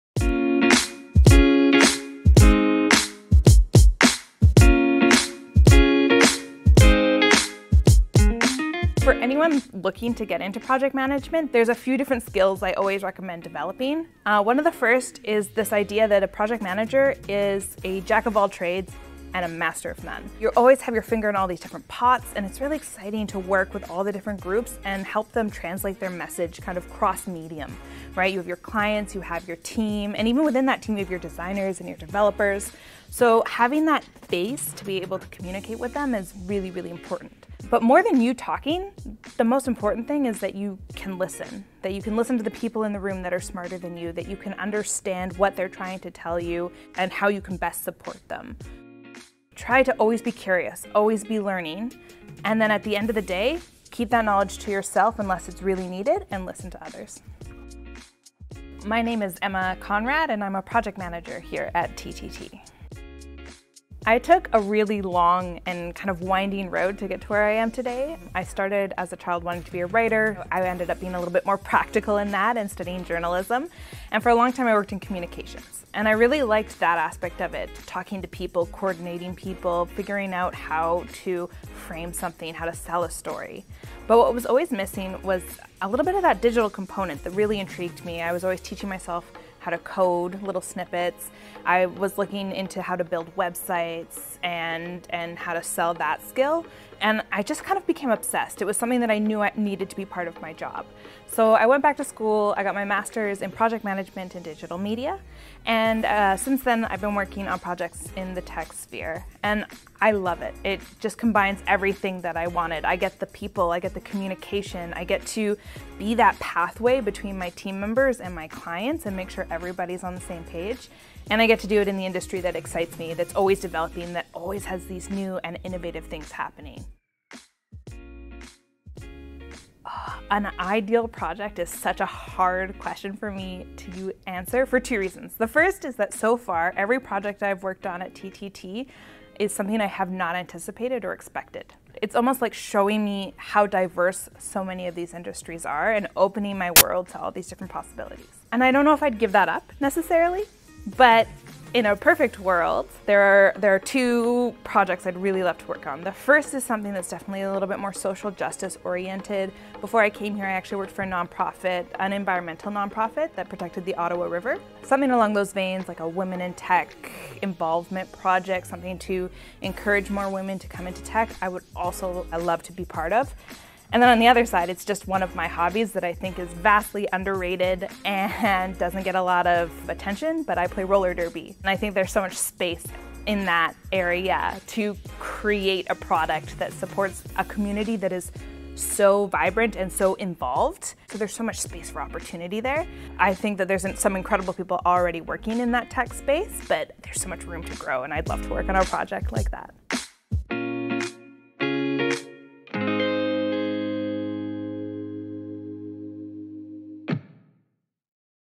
Meet Emma Konrad | Project Manager
In this video, we have a brief conversation with Emma Konrad, our awesome project manager, to get to know her more.

Emma shares the number one thing that has helped her succeed in her role as a project manager at TTT Studios.

Can you guess what's Emma's ideal project? Watch till the end to see what kind of project she would love to be a part of.